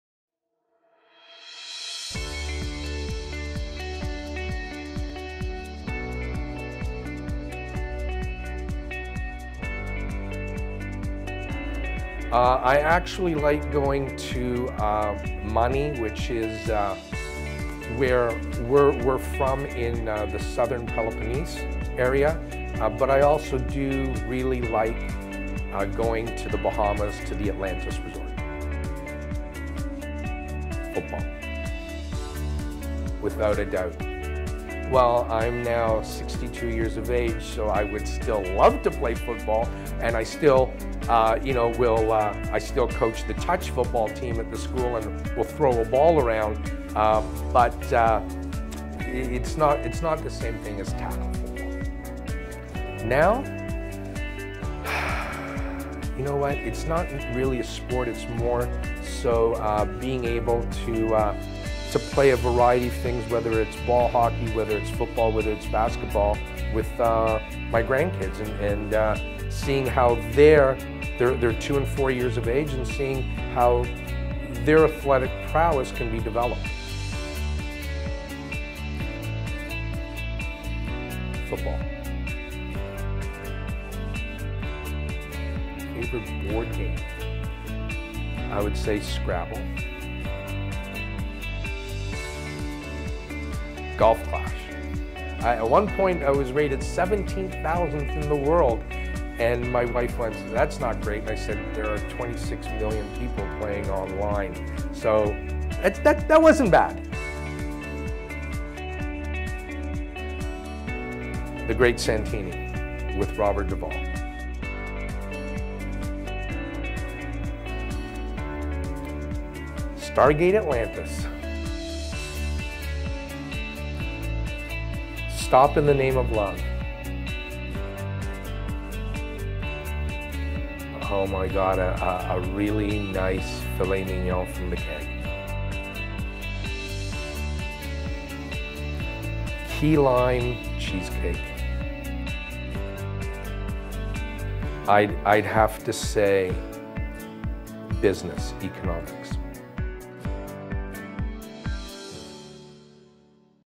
Get to Know Steve Tsimikalis
GetToKnowTPS Principal, Steve Tsimikalis! Steve Tsimikalis (or Mr. T) is co-founder and Principal of the Toronto Prep School (TPS). In this short interview, he answers questions about some of his favourite things.